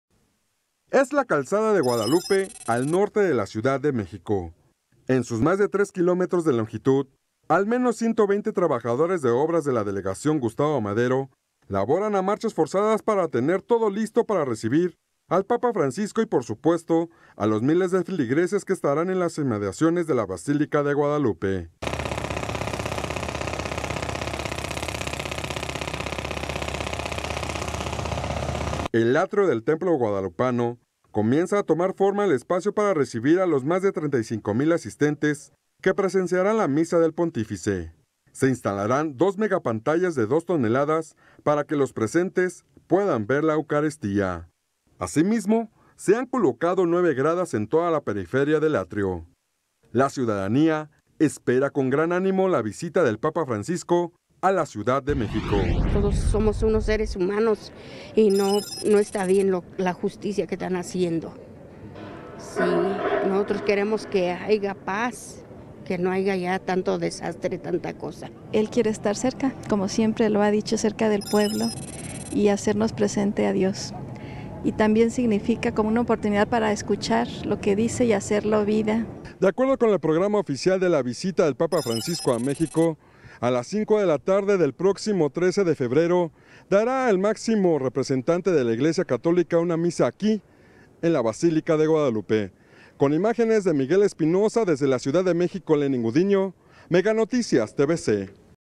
Basílica de Guadalupe, a 10 días de la visita del Papa Francisco
Al menos 120 trabajadores laboran a marchas forzadas para tener todo listo.